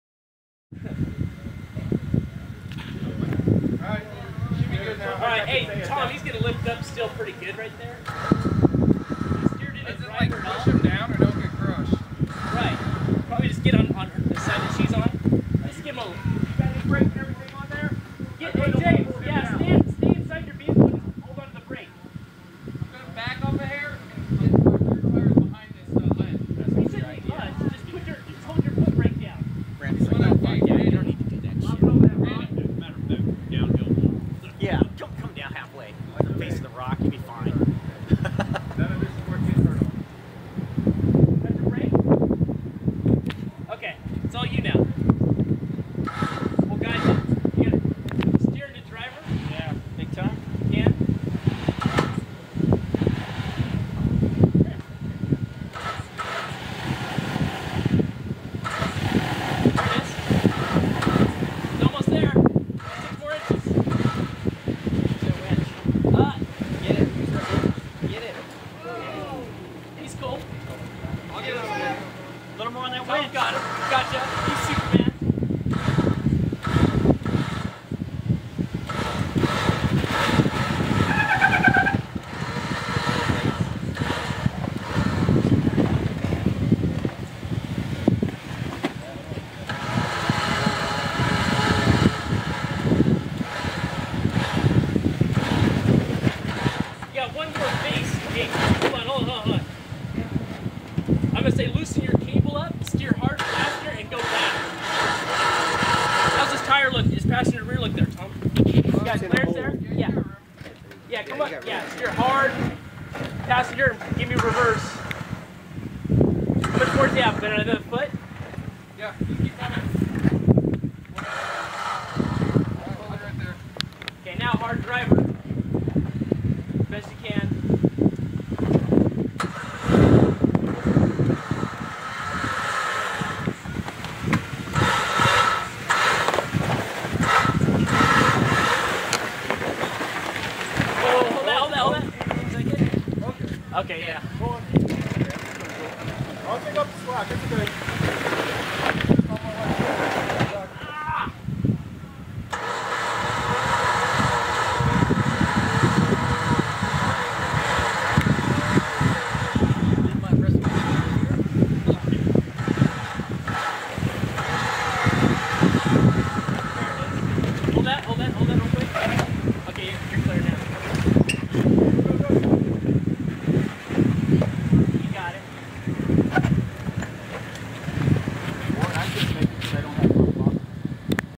Hump N Bump 2011 Shetter Trail-2nd winch of the day
First day of the 31st Hump N Bump in Logandale NV .  This Event is held ever year the first Friday and Saturday of November.